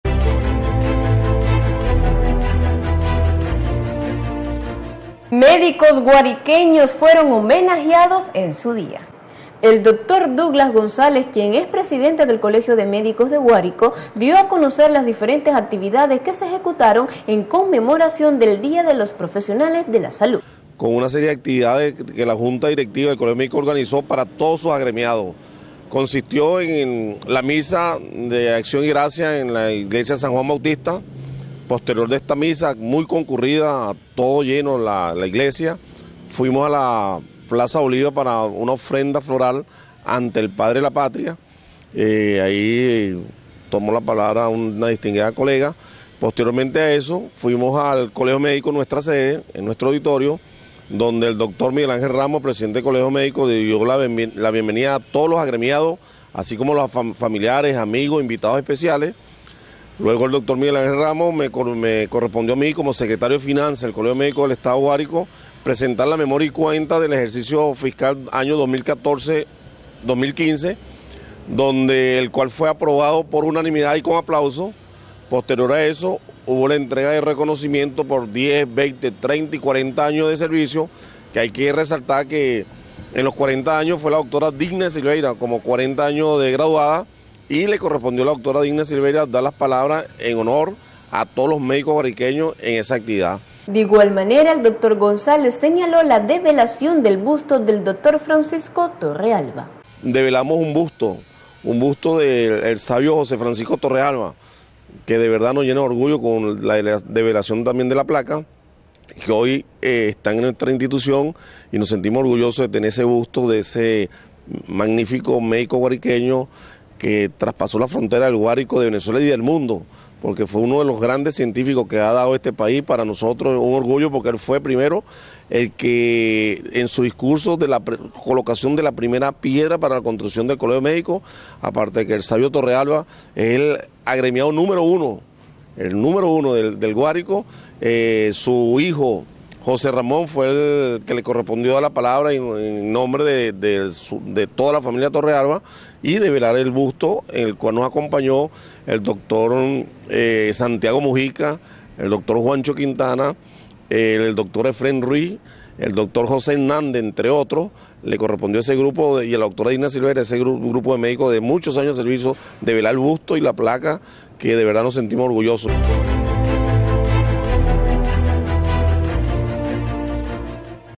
AVANCE N8 MARZO 2015
MÉDICOS GUARIQUEÑOS REALIZARON DIVERSAS ACTIVIDADES EN SU DIA